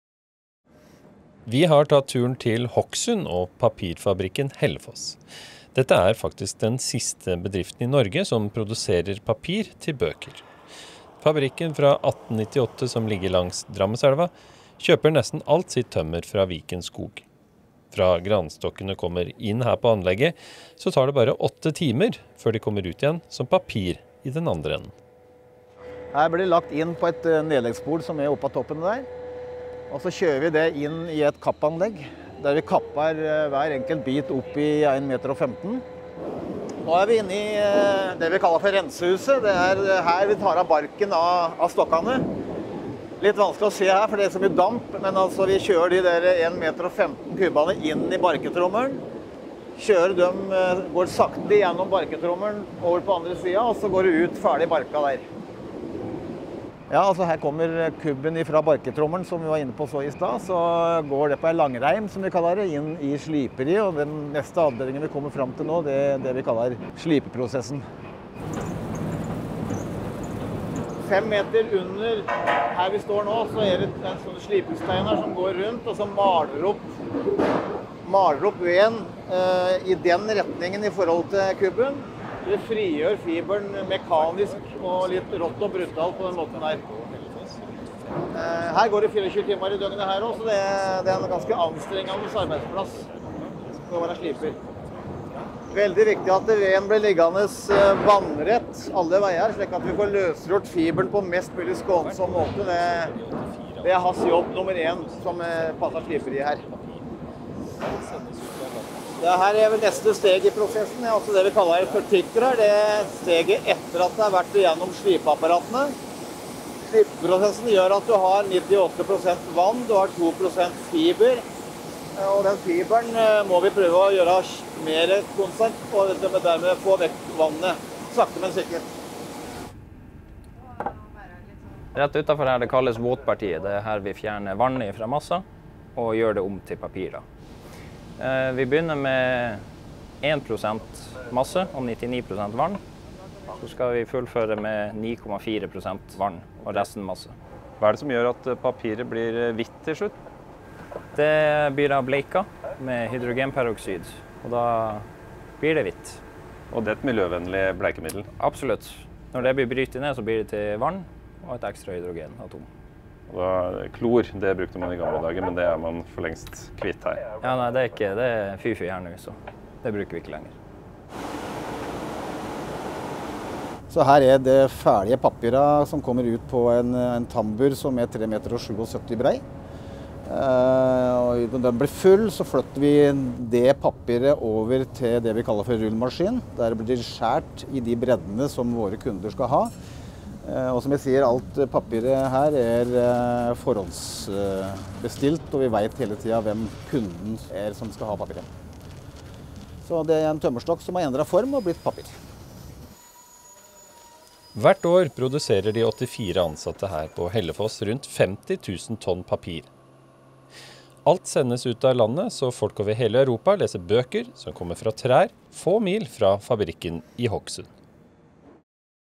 Slik lages bokpapir hos Hellefoss Paper
Hellefoss Paper er den siste bedriften i Norge som produserer papir til bøker.
Fabrikken fra 1898 som ligger langs Drammenselva kjøper nesten alt sitt tømmer fra Viken Skog.
Fra granstokkene kommer inn på anlegget her så tar det bare åtte timer før de kommer ut som papir i den andre enden.
Alt bokpapiret eksporteres.

Laget av Egil Granum, Viken Skog SA.